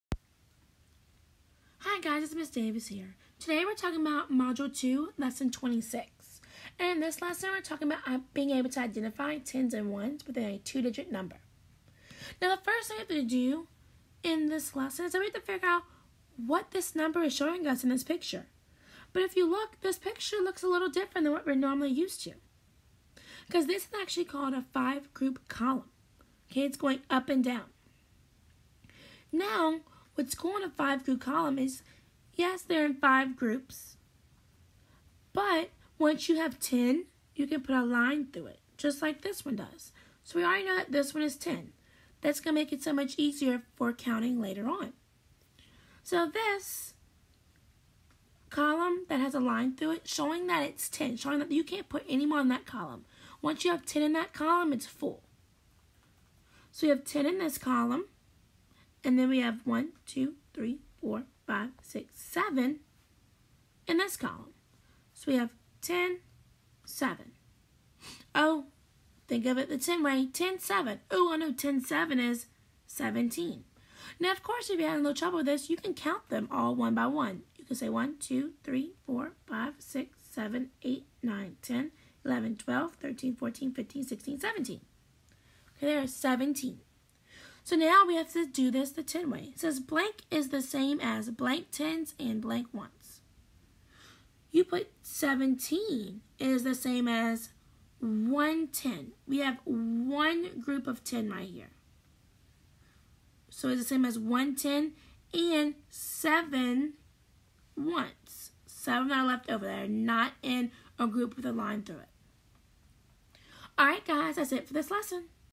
Grade 1 Module 2 Lesson 26
This video shows how to identify tens and ones within a two digit number using Eureka Math Curriculum.